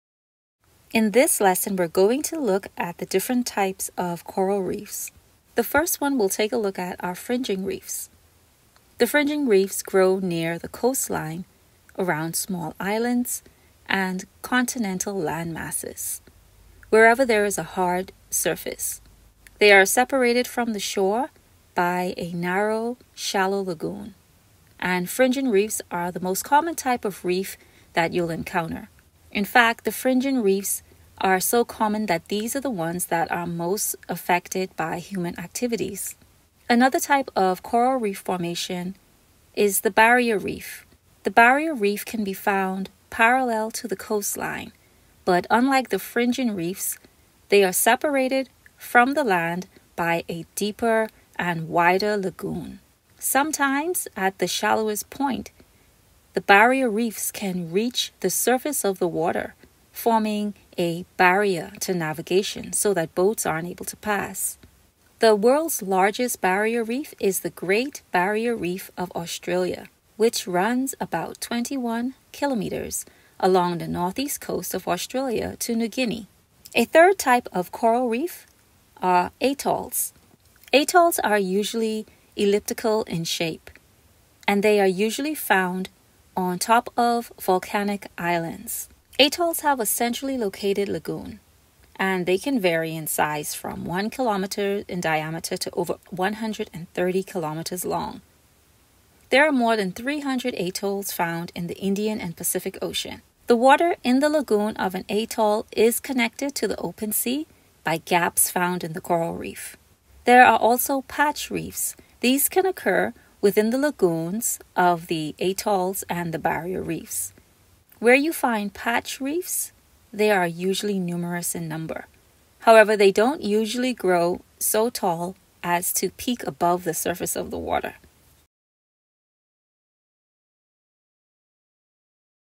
Types of Coral Reefs | Explore Marine Biology
In this video, we will take a look at the 4 types of coral reefs commonly found around the world’s oceans.